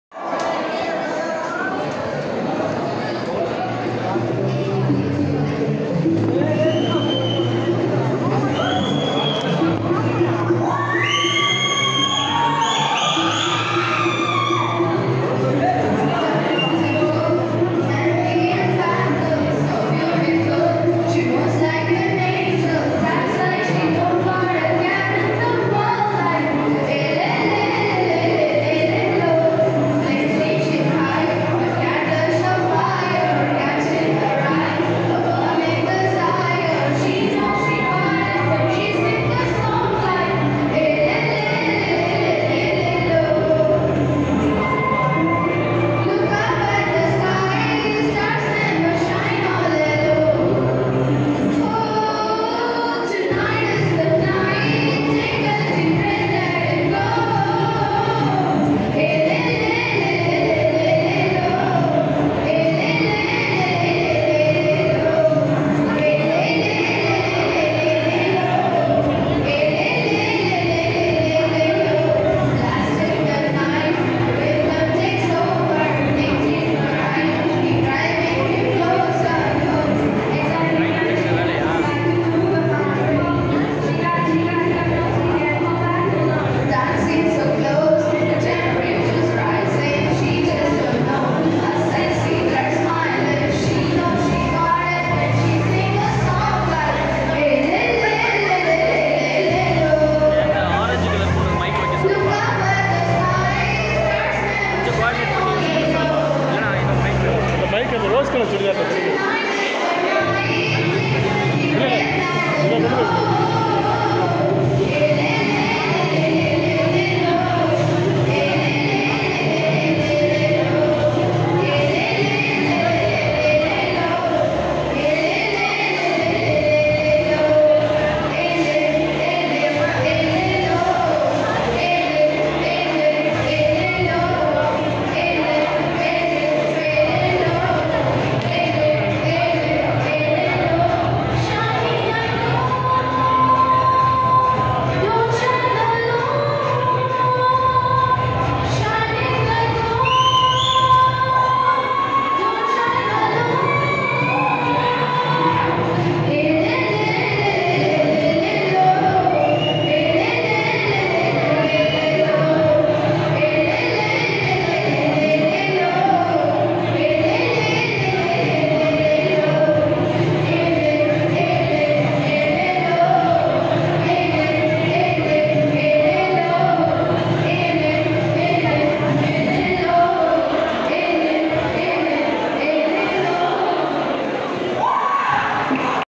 Students from GTN Arts College performed Carol Singing
Carol Singing